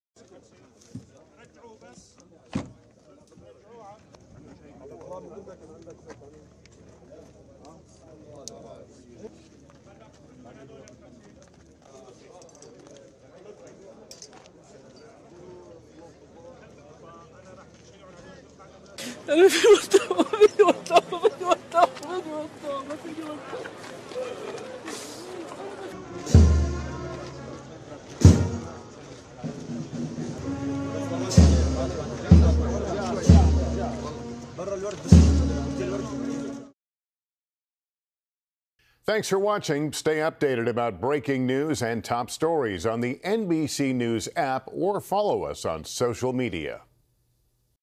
Syria mourns scores killed in drone attack on Homs military academy
Syria on Friday held funerals for scores of people killed in a drone attack on a graduation ceremony at a military academy in the Homs region the previous day, one of the bloodiest strikes against the military in more than 12 years of civil war.